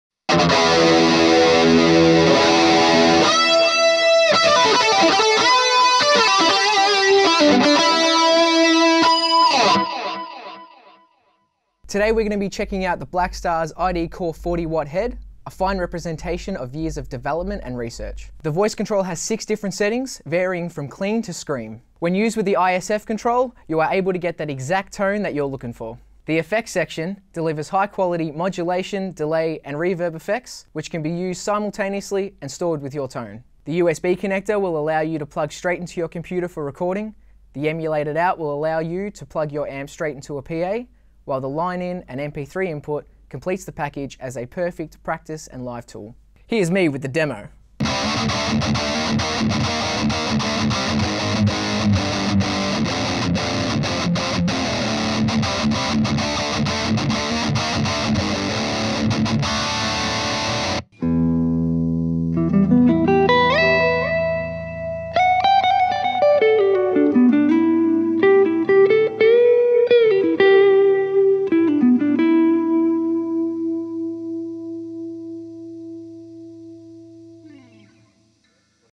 Blackstar ID:Core 40H 40-watt Stereo Guitar Amplifier Head
This Blackstar ID: Core 40w Head is a feature-packed portable amplifier head. You get six amplifier voicings that will cover everything from smooth ballads to heavy, down-tuned metal. And once you sprinkle in a dose of effects, the sound you hear in your head will come out of your amp.